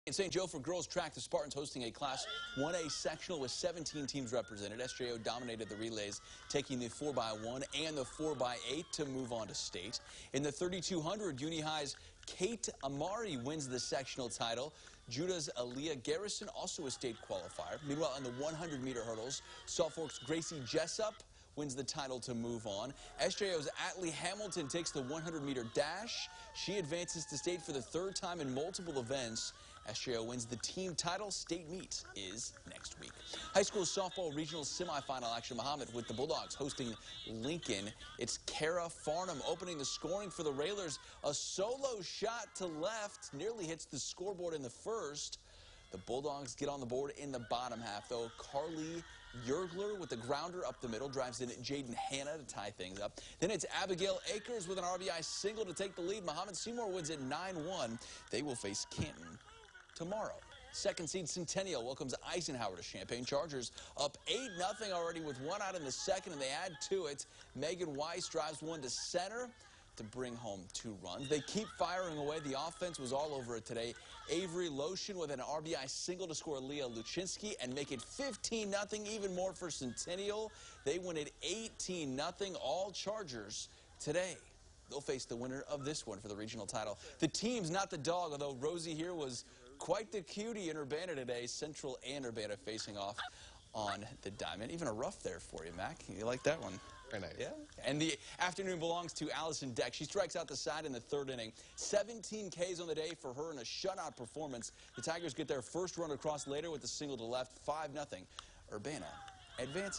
HS scoreboard (6-2-21)
WCIA -- Highlights and scores from high school sports on Wednesday night including postseason softball wins from Urbana, Centennial and Mahomet-Seymour, along with video from the Class 1A St. Joseph-Ogden girls' track and field sectional.